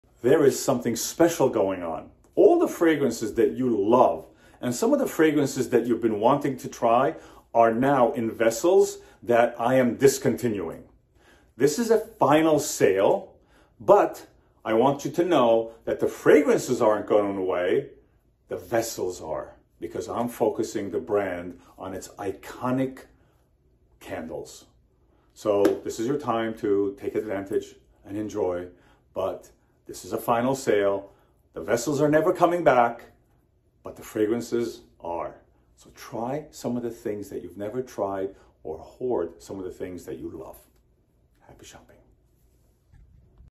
Discontinuation Sale, Let's make room for the future of Thompson Ferrier Candles
I am going to do something I have never done before. I am going to have a DISCONTINUATION SALE.

The sale goes live this Friday June 10 at 8:00 PM EDT. I'm doing this to narrow the brand focus on my iconic candles and fragrances which means that certain other Thompson Ferrier candles are going to be retired at SURPRISING markdowns.

Don't worry though, I won't be retiring my fragrances. This discontinuation only applies to certain vessels.

THIS IS YOUR CHANCE TO GET THE FRAGRANCES YOU LOVE OR TO FINALLY TRY SOME NEW SCENTS AT INSANE MARKDOWNS!

Ready to shop? Head to thompsonferrier.com at 8PM EDT tonight

Recommended video you should check out!

Go and check us out in our other social media channels!